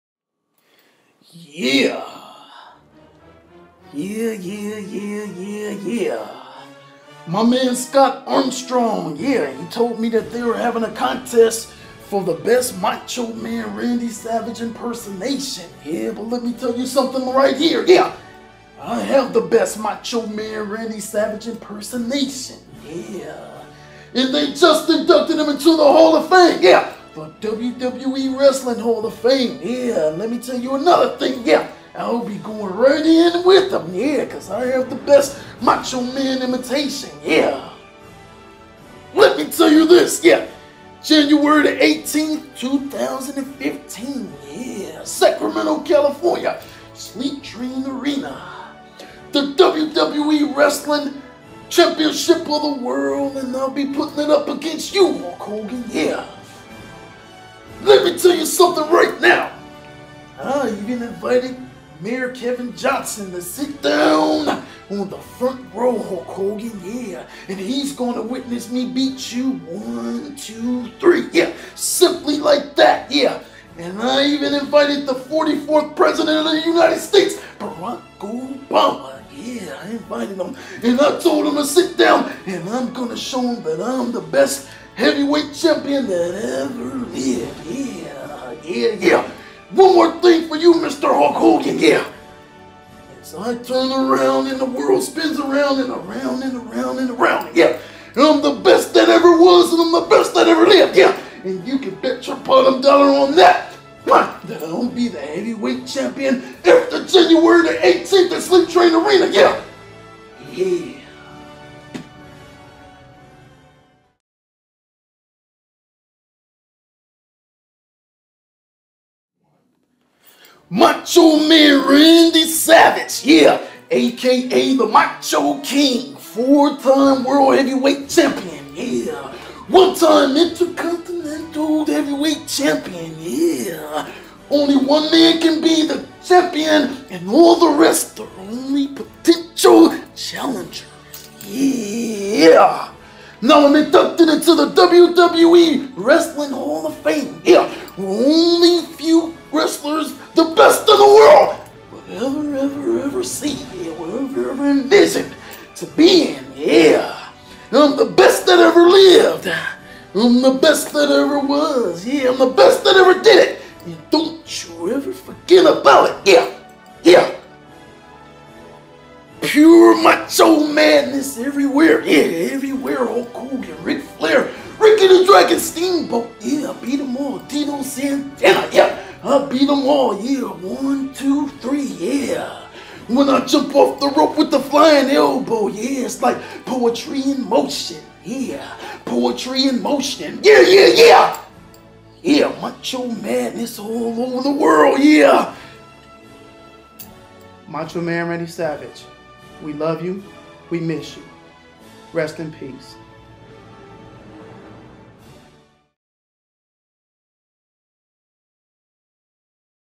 Macho Man Tribute
Terrel The Barber pays tribute to one of the best wrestlers to ever step in the ring. OHHHHH YEEEAAAAHHHH!!!

Terrel Smith
Terrel Smith the barber 916
Barbershop name and location:
CeCe's cutz
4261 Truxel Rd Sacramento, CA